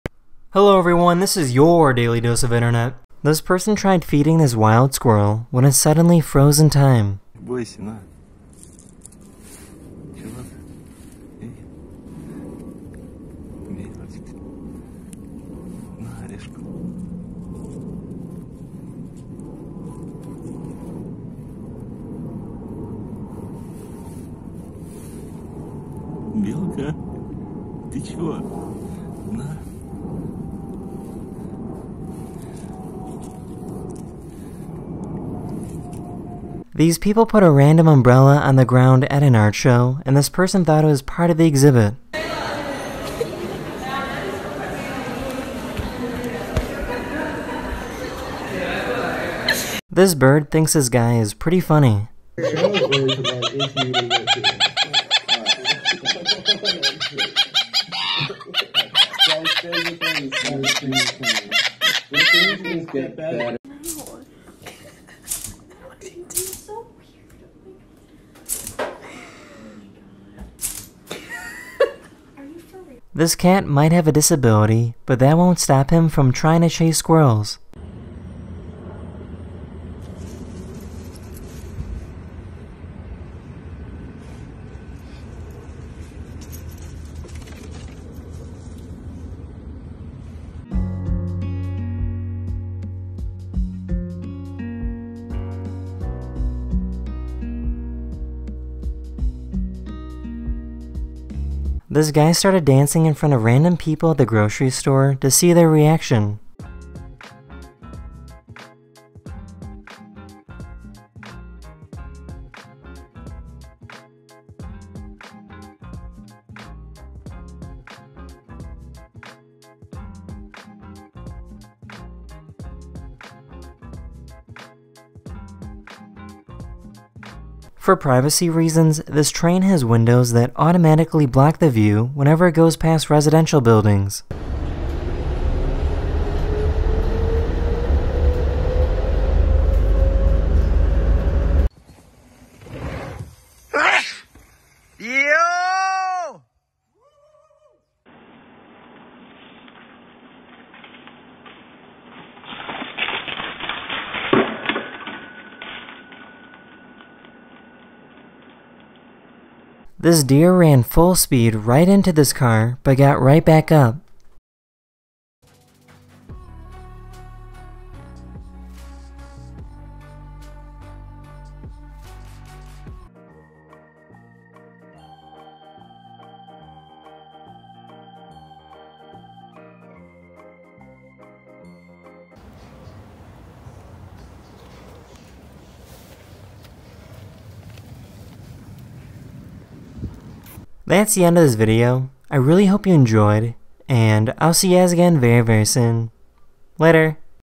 Squirrel Found Frozen In Time
Hello everyone, this is YOUR Daily Dose Of Internet! In this video, a man finds a squirrel that is not moving.

Also in this video, people find an umbrella at an art show. A bird has a funny laugh. A dog has fast quick movement. A cat with a medical condition. Swimming through jellyfish. Spinning on the floor at a grocery store. The window on a train will dim near residential buildings.